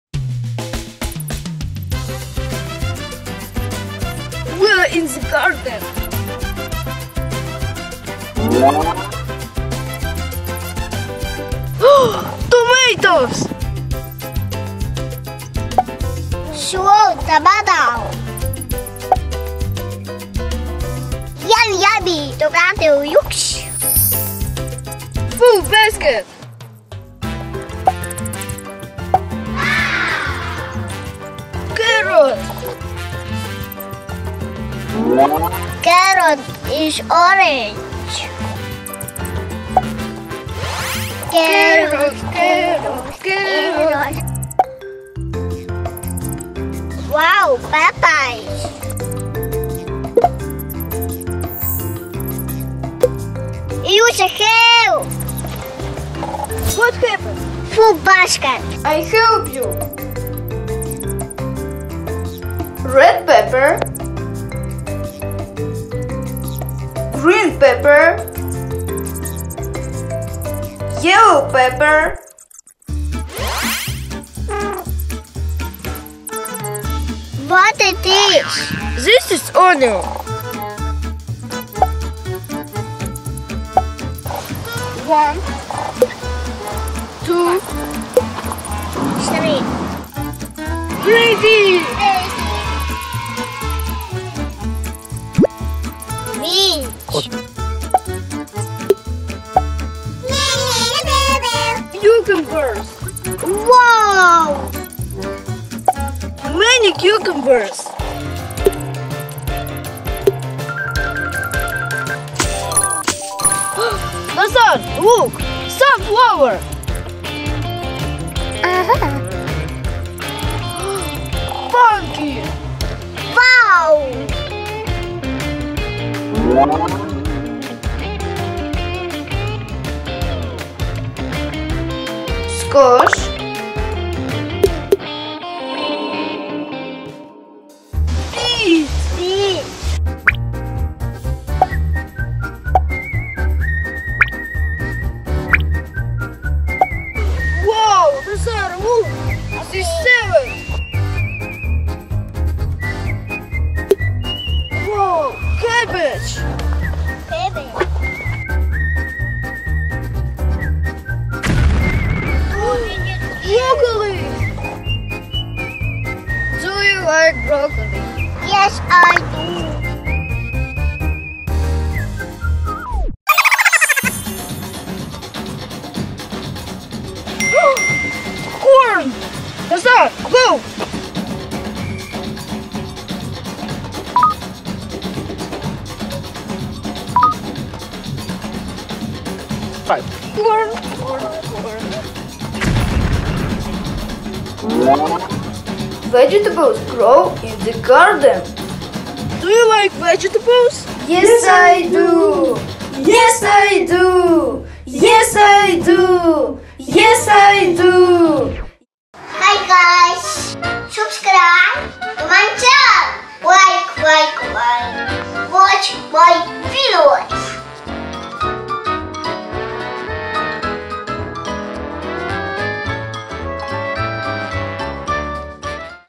Learn to Harvest Vegetables. Playing in the Garden. Educational Video for Kids
Boys Gardening and learn vegetables. Farm vegetables for kids. Educational outdoor game in the garden.

Join Nazar and his family on this channel for fun adventures, learning and songs.